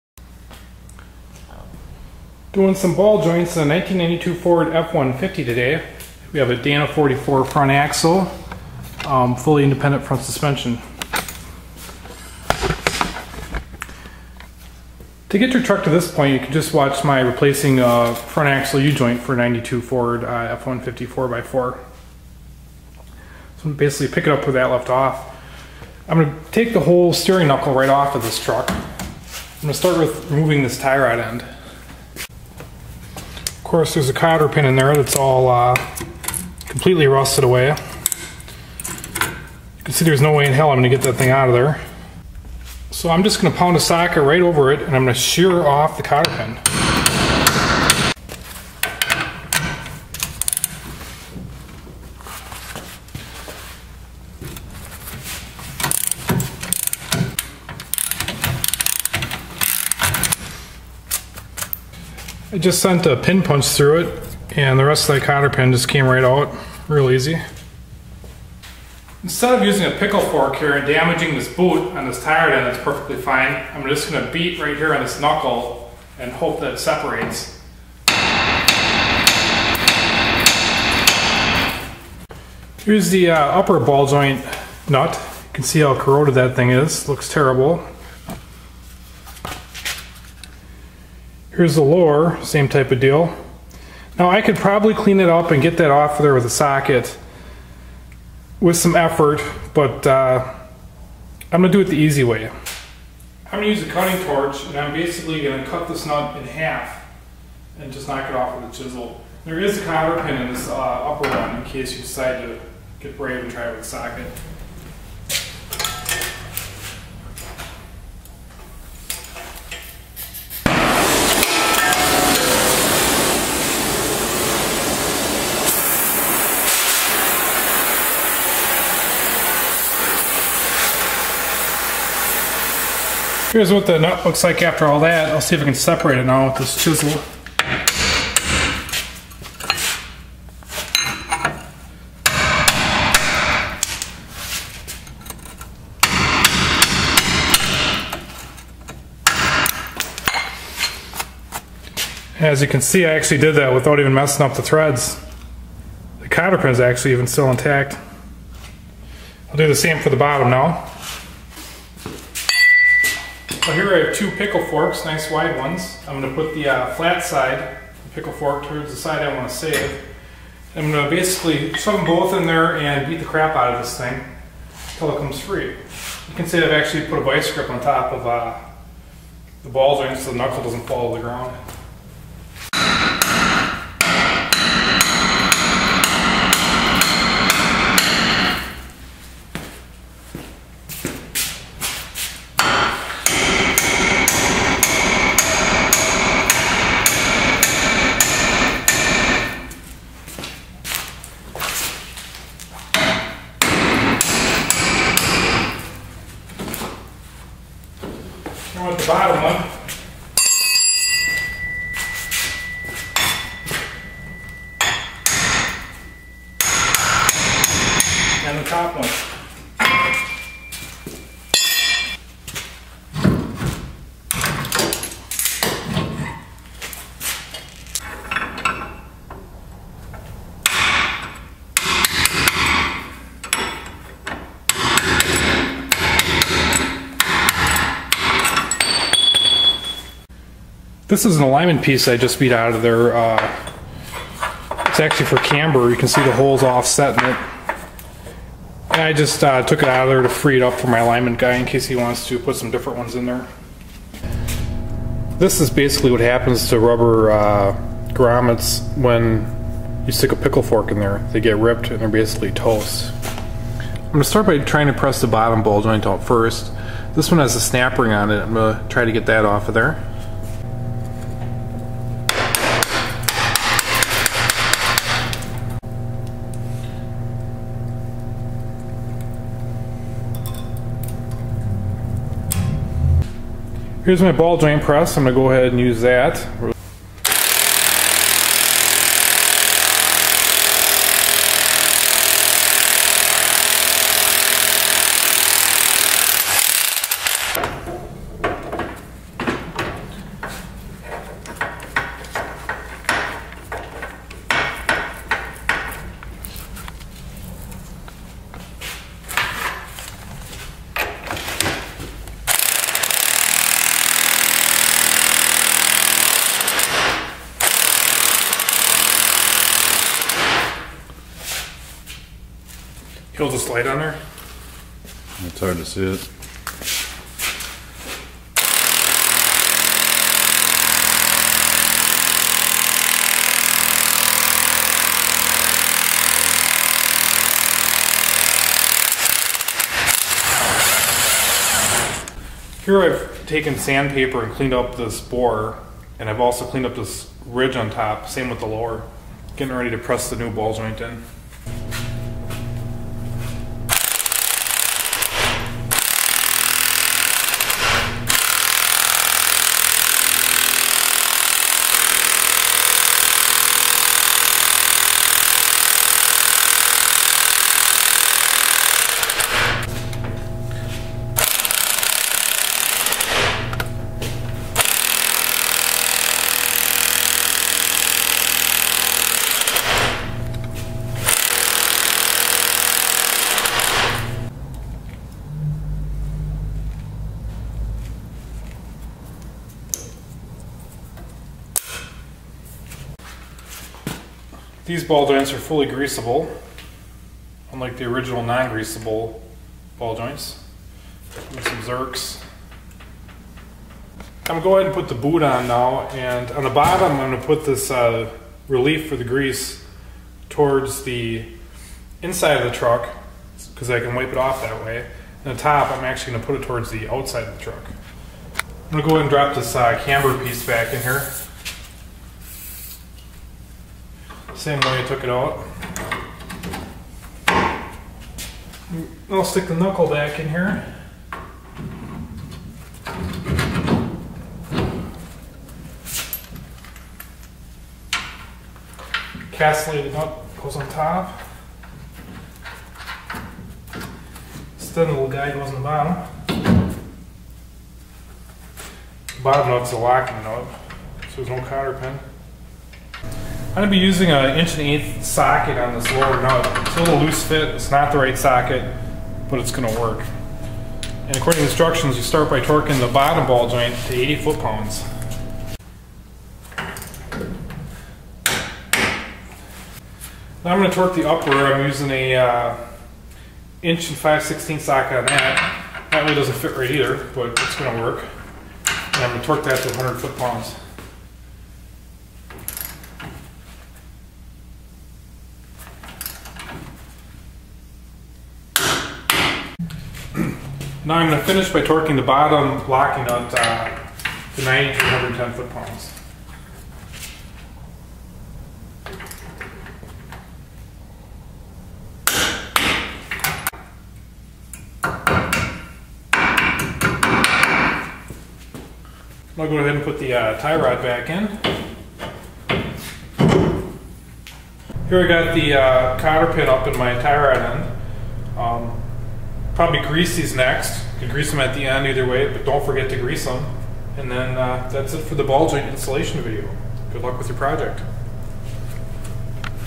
Ball joint change in a 1987 - 1996 Ford F150 or Bronco 4x4
Changing out the ball joints in old crusty. Some rust belt tips are given in the video. It's possible that this is similar to 1987 to 1996 f150 pickups and Broncos with the ball joints integral to the knuckle.  Dana 44 IFS There are two sister videos to this, one on a f150 front axle u-joint replacement and another on camber bushing replacement.